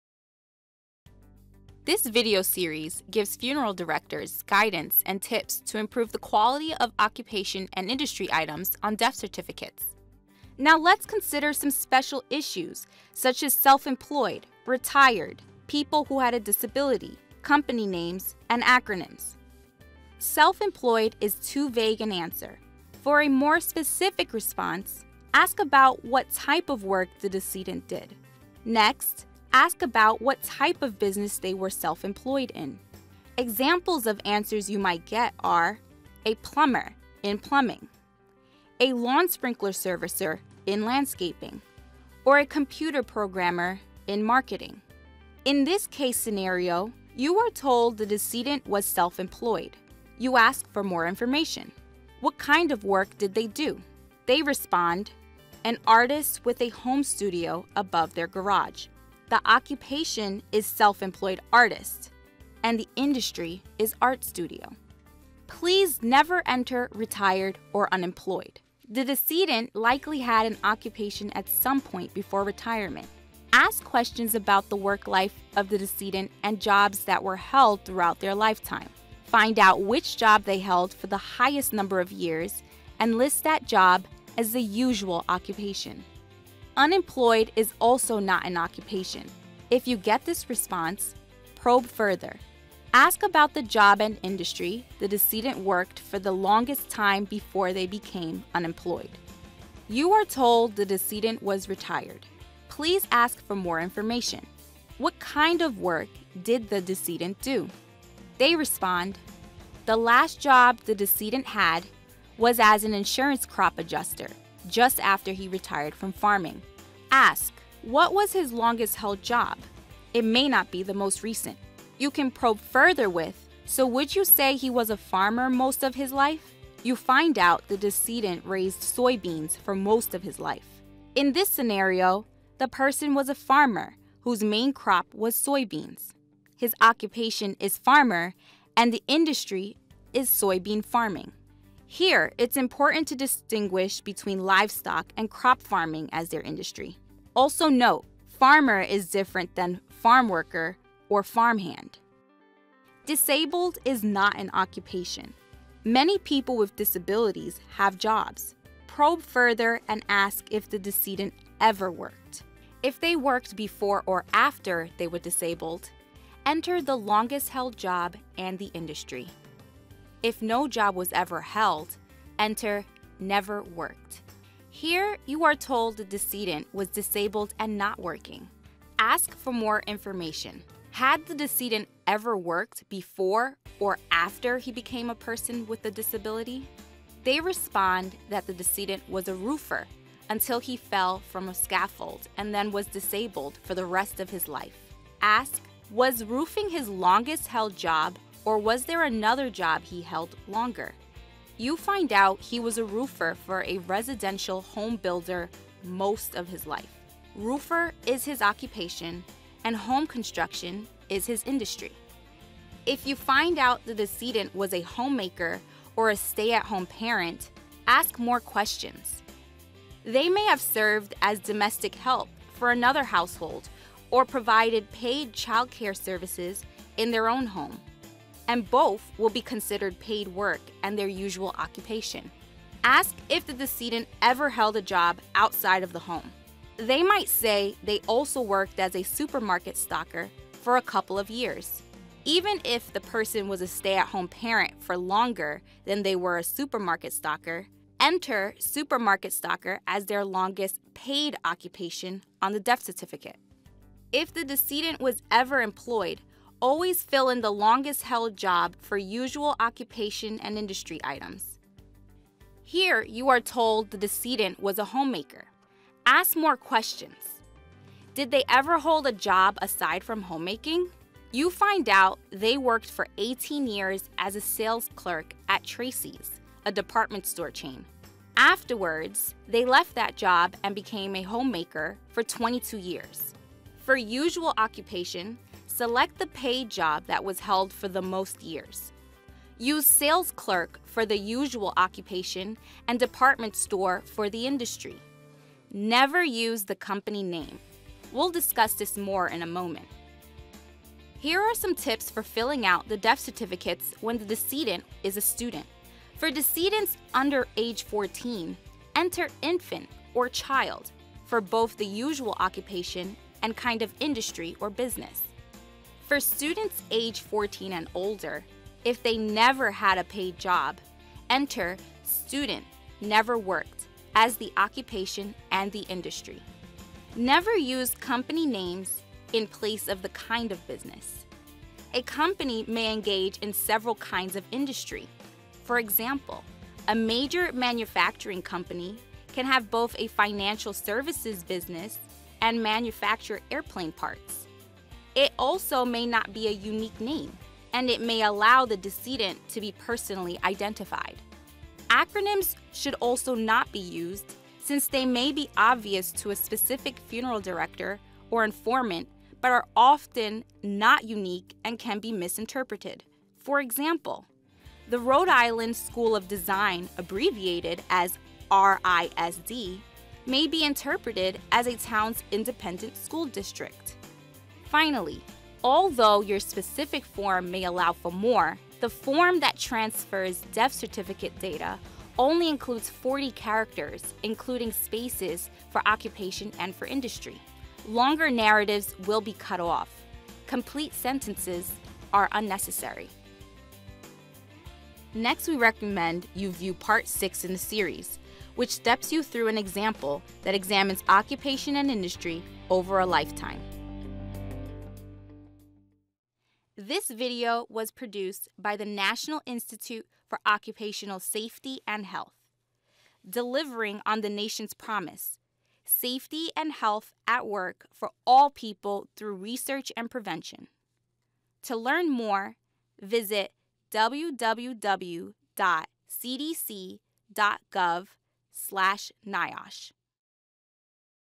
Recording Industry and Occupation on Death Certificates Video Series Part 5
Part 5: Special Issues: Self-employed, Retired, People Who Had a Disability, Company Names, and Acronyms
Part 5 reviews special circumstances and how to address them. Specifically, we discuss documenting usual occupation and industry for people who were self-employed, retired, or had a disability. We also discuss why you shouldn’t record company names or acronyms.